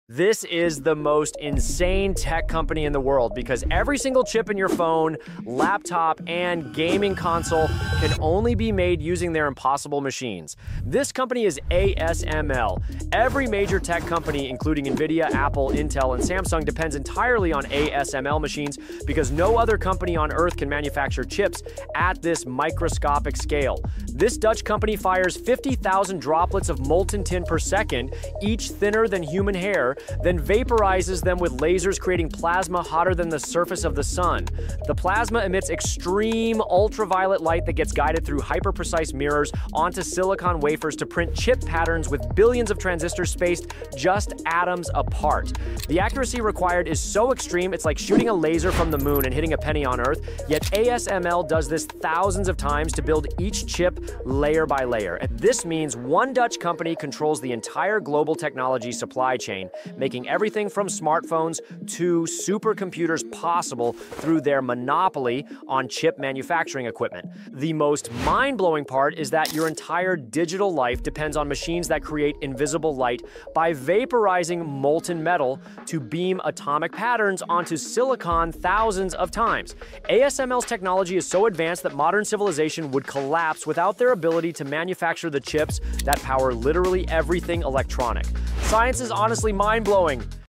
THIS COMPANY CONTROLS ALL TECH COMPANIES
💥 Every chip in your phone, laptop, and console depends on one insane machine—built by ASML.

✅ Fires 50,000 molten tin droplets/sec
✅ Lasers vaporize them into plasma hotter than the sun
✅ Prints atoms onto silicon with space-age precision
✅ Powers Nvidia, Apple, Intel, Samsung—everyone

Your digital life runs on invisible light and molten metal.